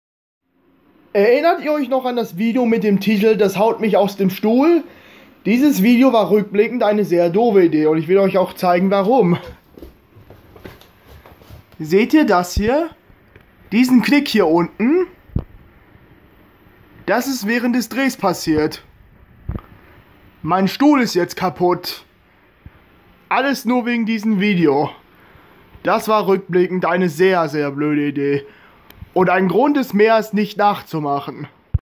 Eine ganz dumme Idee
Das Video ,,Das haut mich aus dem Stuhl " war eine blöde Idee.